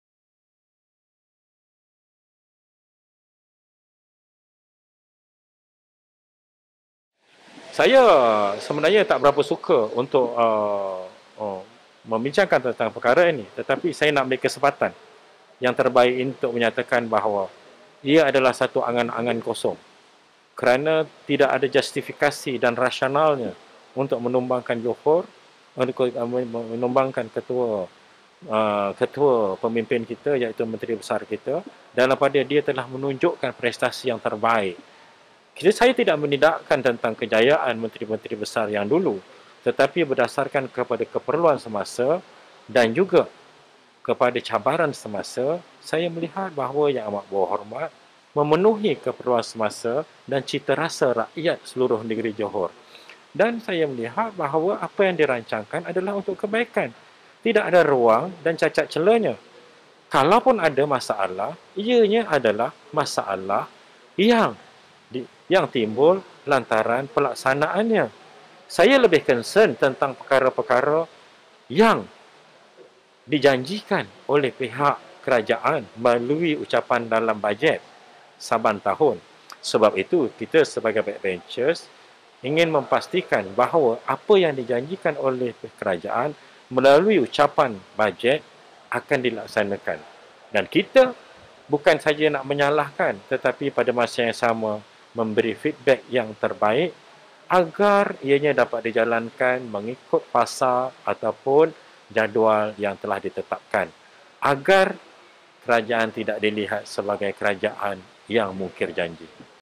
20160420 Gerakan Menjatuhkan MB Johor Adalah Angan-Angan Kosong - YB Dato' Samsol Bari
Gerakan Menjatuhkan MB Johor Adalah Angan-Angan Kosong

YB Dato' Samsol Bari
ADUN Semarang N20